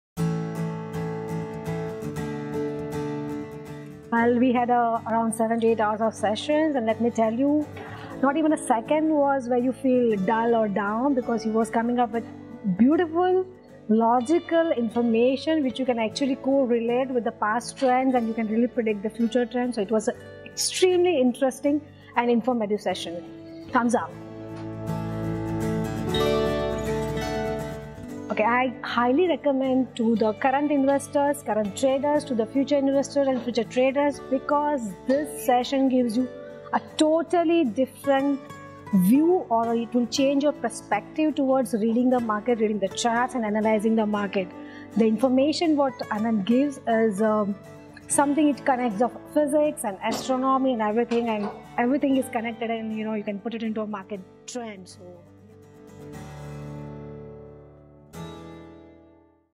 Interesting & Informative Session | Anant Acharya | Stock Market
The workshop made Teena change her perspective towards reading the charts & analyzing market trends.📉📊

Watch to know her learnings from the session.